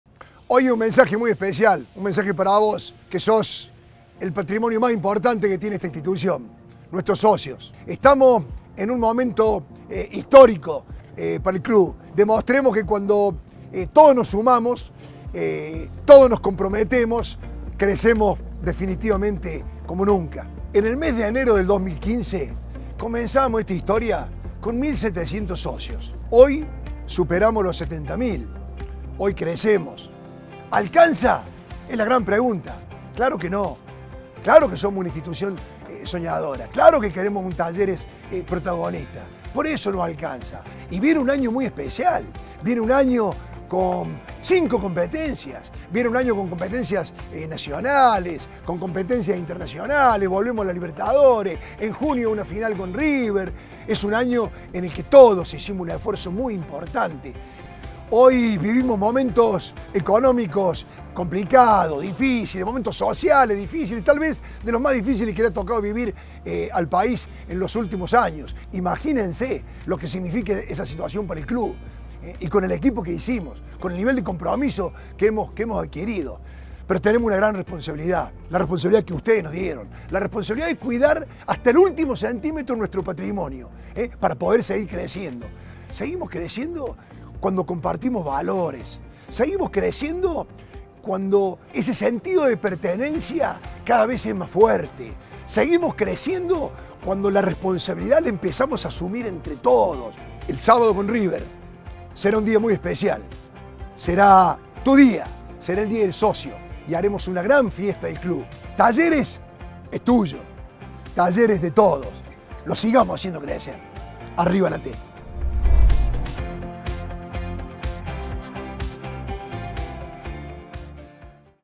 ¡Se viene el DÍA DEL SOCIO y vivimos la 𝗙𝗜𝗘𝗦𝗧𝗔 𝗗𝗘𝗟 𝗖𝗟𝗨𝗕! 🔵⚪
El DÍA DEL SOCIO es un partido en el cual cada uno de nosotros que 𝗾𝘂𝗶𝗲𝗿𝗲 𝘆 𝘀𝗲 𝘀𝗶𝗲𝗻𝘁𝗲 𝗼𝗿𝗴𝘂𝗹𝗹𝗼𝘀𝗼 𝗱𝗲𝗹 𝗧𝗮𝗹𝗹𝗲𝗿𝗲𝘀 que tenemos 💙

¿Cómo hago para estar presente?

💻 Ingresá en 𝘄𝘄𝘄.𝗯𝗼𝗹𝗲𝘁𝗲𝗿𝗶𝗮𝘃𝗶𝗽.𝗰𝗼𝗺.𝗮𝗿/event/description/753 y con tu usuario podés abonar el aporte excepcional.

🏟️ El sábado presentá tu carnet físico con el abono realizado e ingresás a la 𝘁𝗿𝗶𝗯𝘂𝗻𝗮 𝗱𝗲𝘀𝗶𝗴𝗻𝗮𝗱𝗮.

Este mecanismo se aplicará extraordinariamente en el duelo de la Fecha8 ante River Plate por la 𝗖𝗼𝗽𝗮 𝗱𝗲 𝗹𝗮 𝗟𝗶𝗴𝗮 Sur Finanzas 🏆